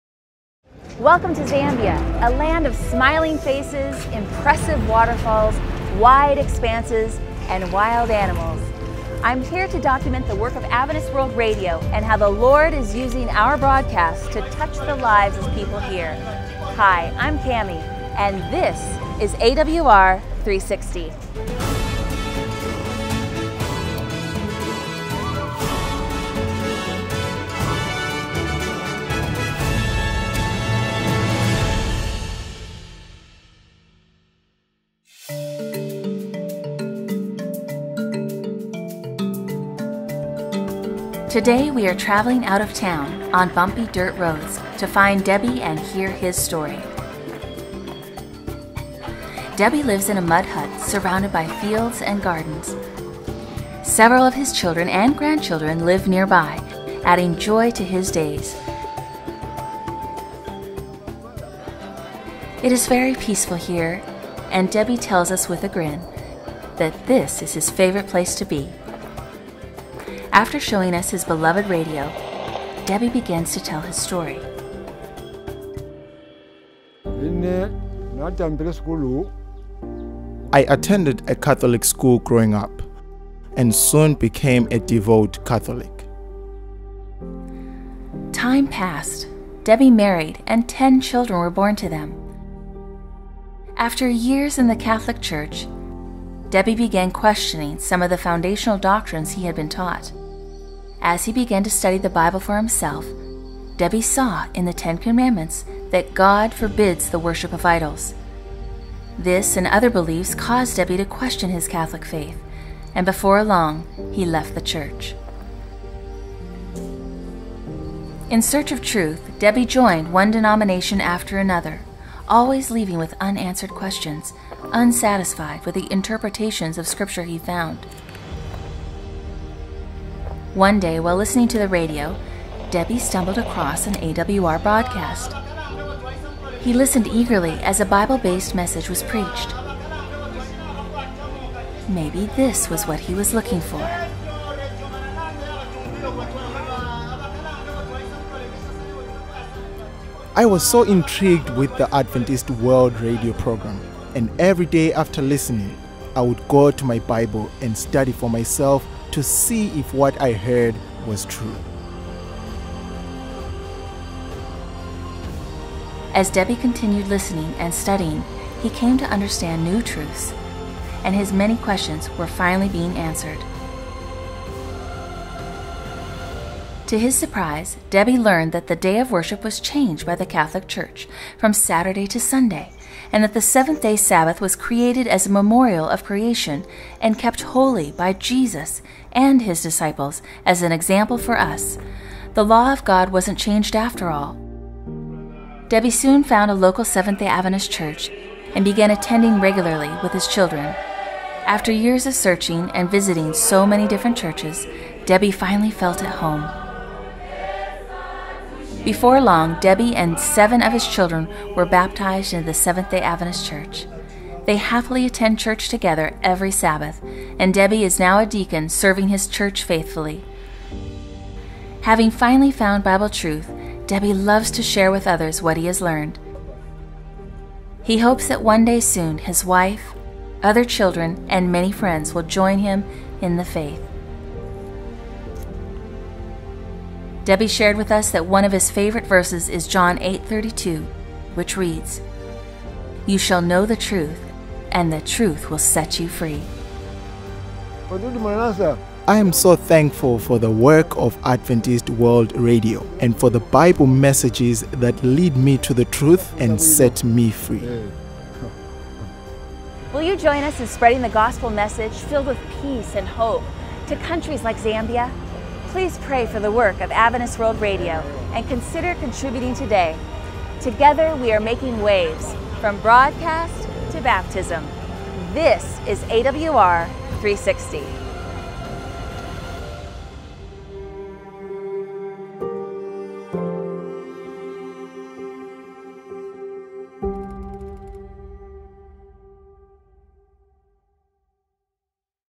AWR360° Zambia - In Search of Truth. Debby's Story | Miracle Story
Although a Christian for many years, Debby had many unanswered questions about his faith.

See how God used the airwaves to help him get answers to these questions and change his life